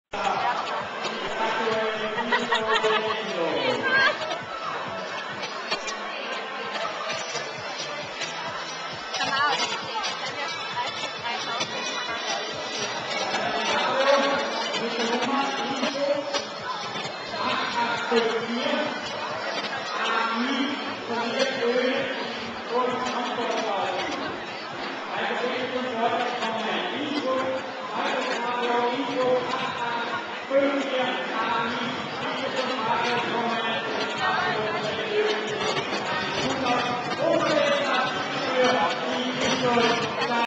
42. Seefelder Blumencorso
Am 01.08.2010 wurde der 42. Blumencorso in Seefeld in Tirol veranstaltet. Rund 80 Fahrzeuge präsentierten sich bei bestem Wetter unter dem Motto "FESTA DO BRAZIL" den zahlreichen Gästen und Zuschauern.